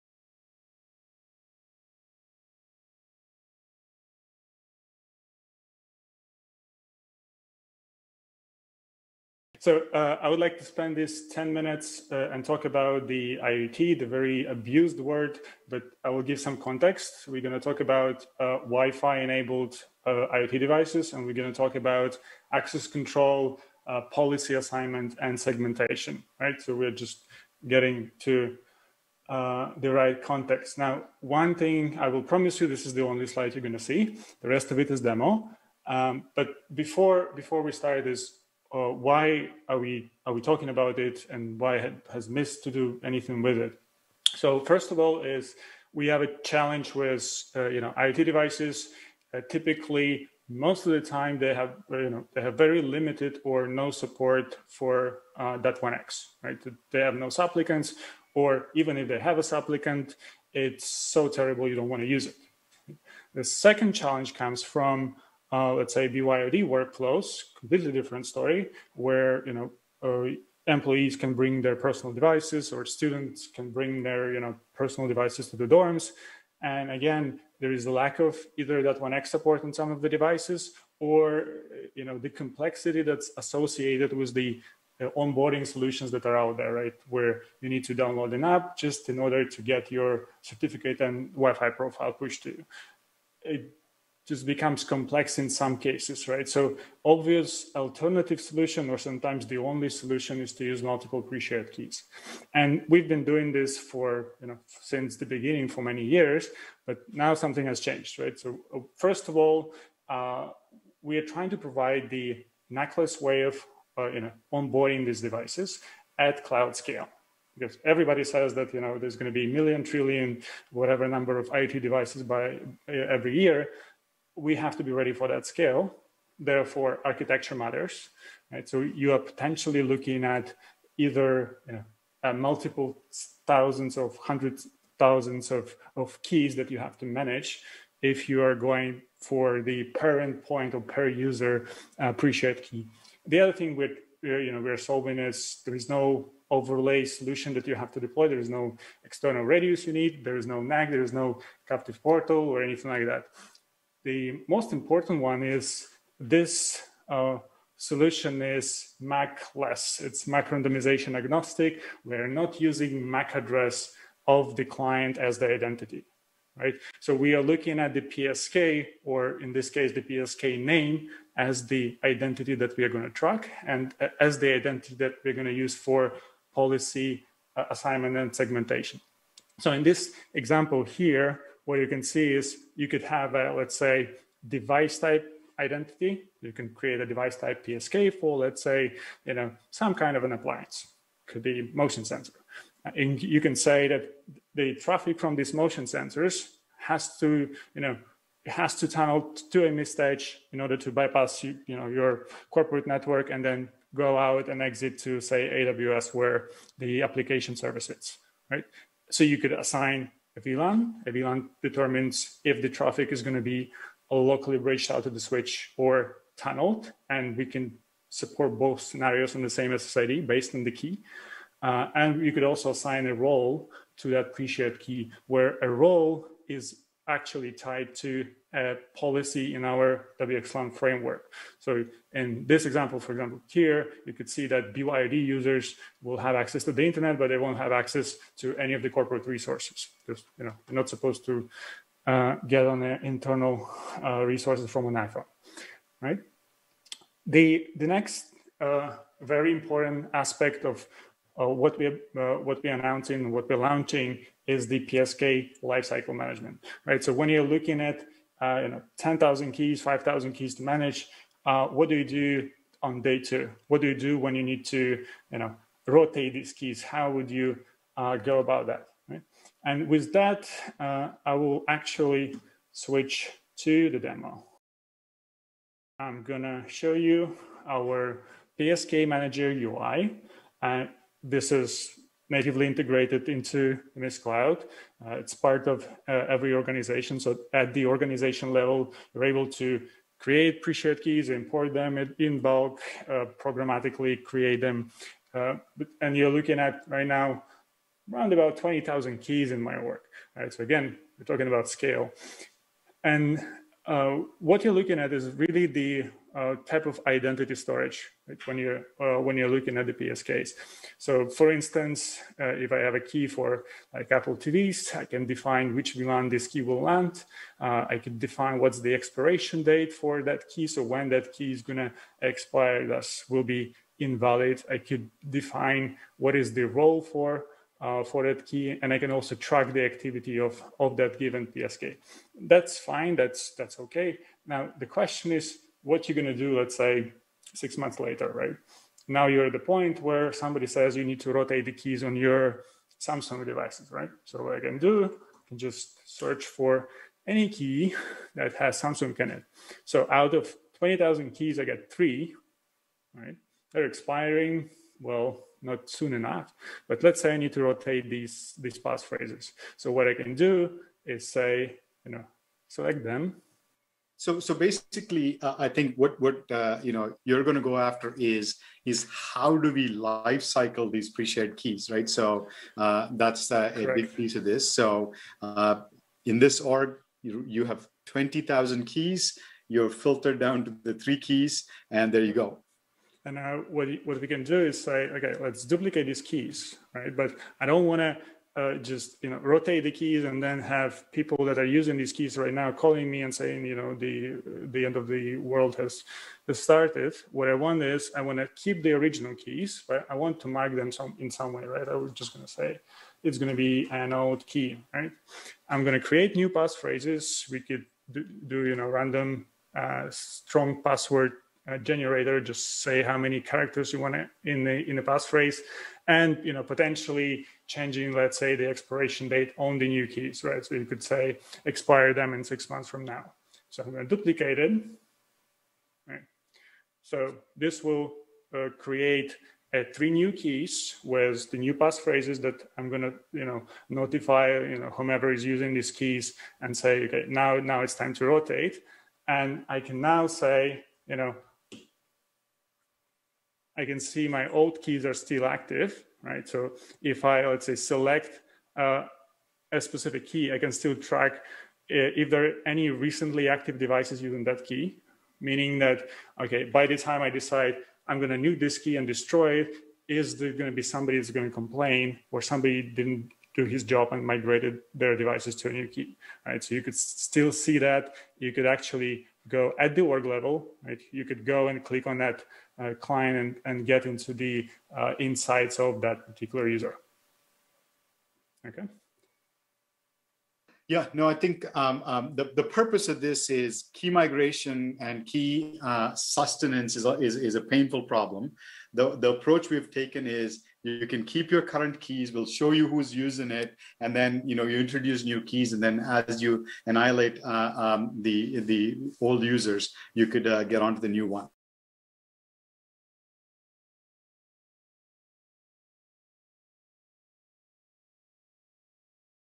Juniper Mist IoT at Scale Demo
As IoT devices continue to soar in customer networks you need to learn how to scale the provisioning, policy and visibility for these devices. Slava will walk through how we are helping customers scale their IoT deployments through a live demo.

Presented by Slava Dementyev, Sr. Product Manager. Recorded at Mobility Field Day 6 on July 15, 2021.